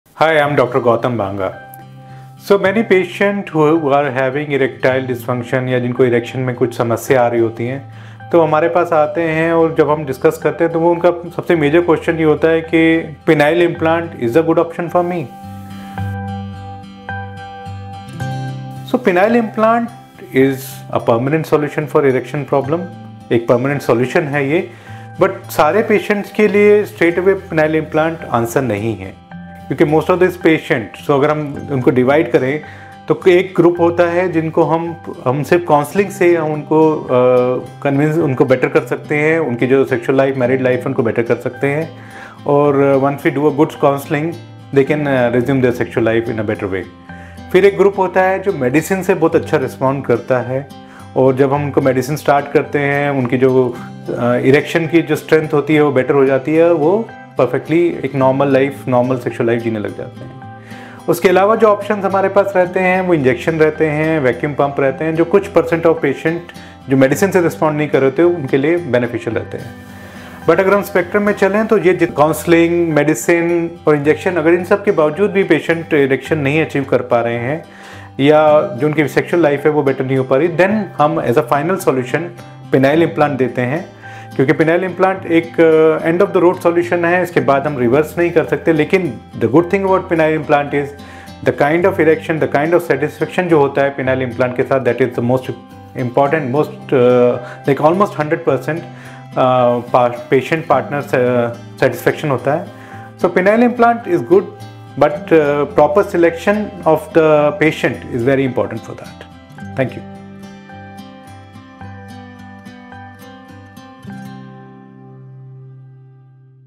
Best Penis implant surgeon, hospital and treatment in India
Ask the Expert: What is penile implant surgery?
Penile implant surgery involves placing a prosthetic device inside the penis to enable erections in men with erectile dysfunction. There are two main types: inflatable and malleable implants. The procedure is typically considered when other treatments are ineffective. It aims to restore sexual function and improve quality of life. Consultation with a specialist is essential.

we are available on other platforms. You can follow
Whats UP - UPS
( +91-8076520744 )

or visit SCI International Hospital, GK-1, Delhi